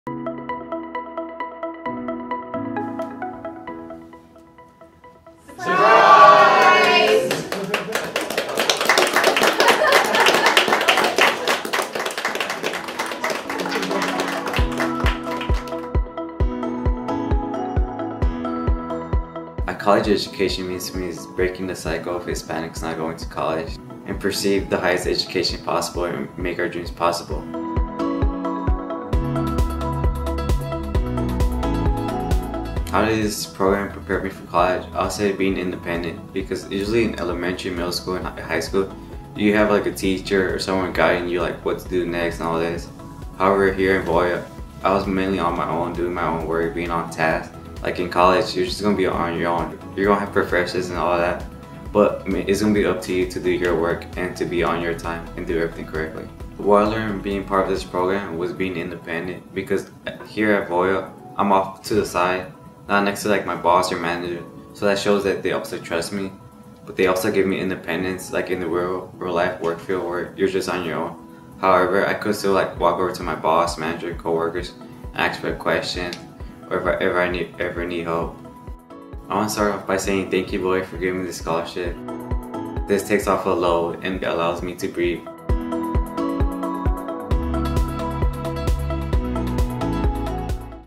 2019 Voya Scholars Recipient Jonathan Esquivel – Atlanta, Georgia
Jonathan is a first-generation college student and a former Voya intern through Cristo Rey High School’s corporate work study program. Jonathan says that he hopes to bring awareness around education within the Hispanic community, and talk to his peers about the possibilities that furthering education can bring. Although he is still deciding on a major, Jonathan will be attending Oglethorpe University in the fall.